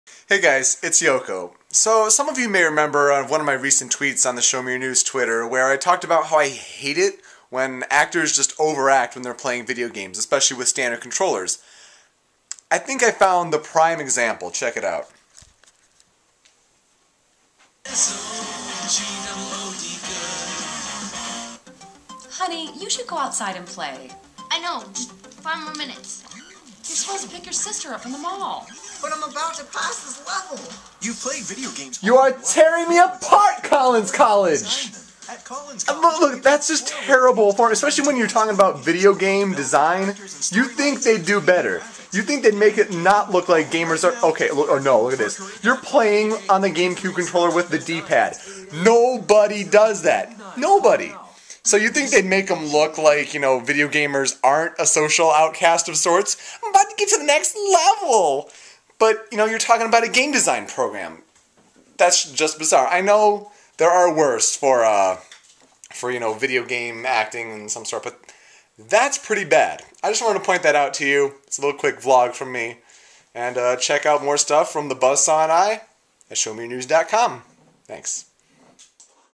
Collins College Ad Fail
Pay attention to the controllers. This isn't how you advertise to video gamers to come to your school.